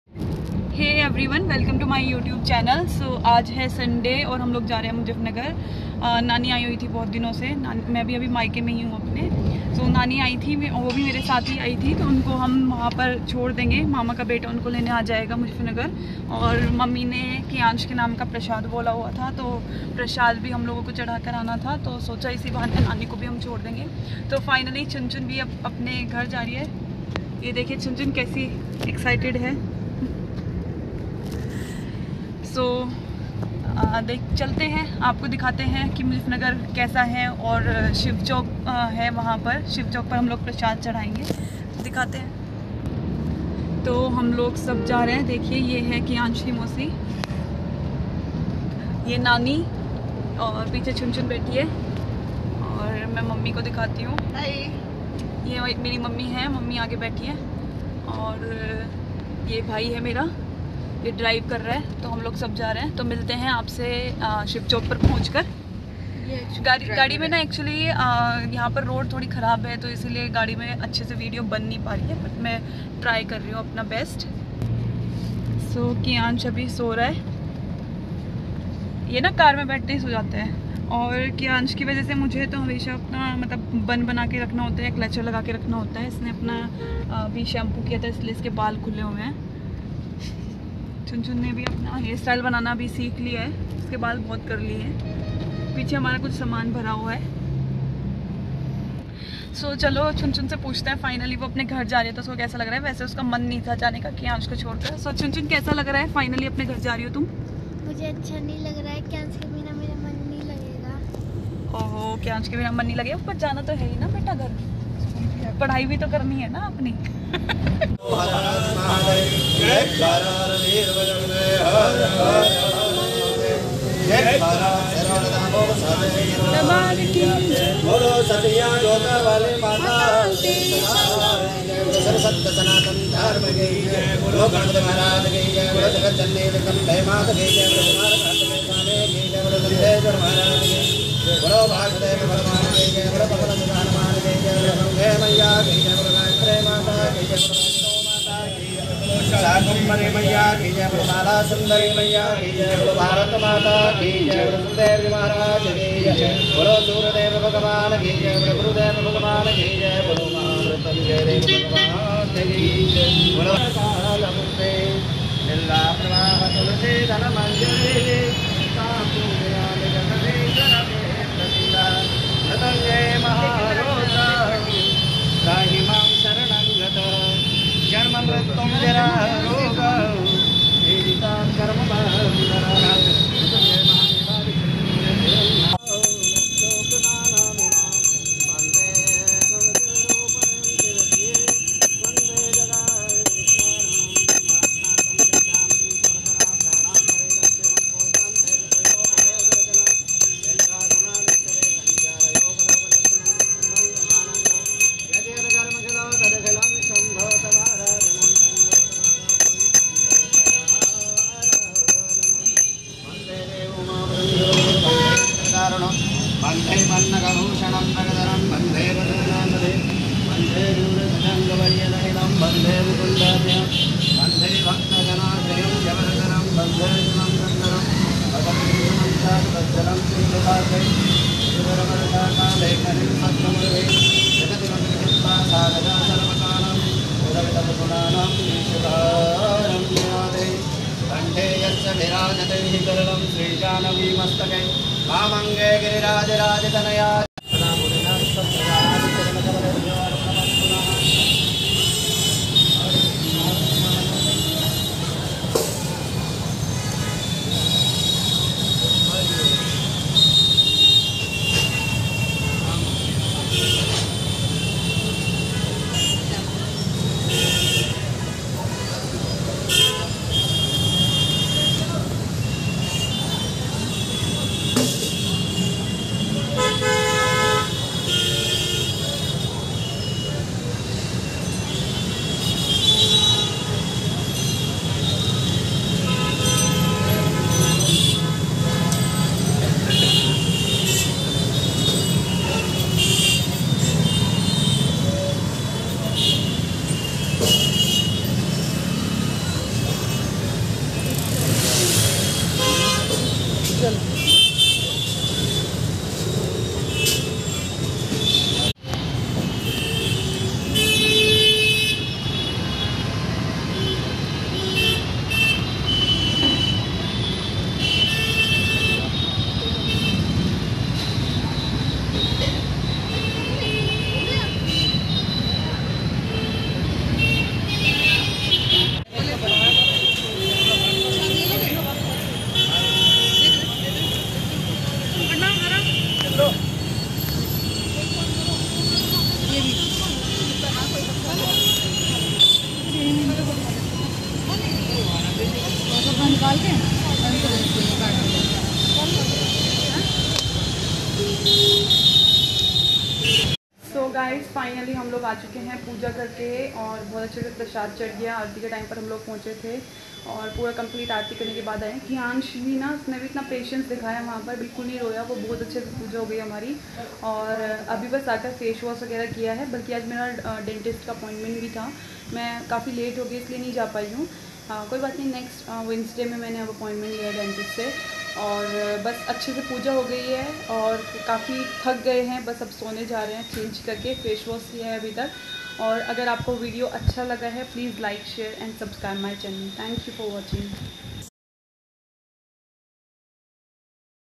Hum gye Kiyansh ka prasad chadhane Shiv Chowk Mandir, Muzaffarnagar
Hum gye Kiyansh ka prasad chadhane Shiv Chowk Mandir/ Muzaffarnagar Shiv Chowk Mandir/ Kiyansh's first visit to Muzaffarnagar.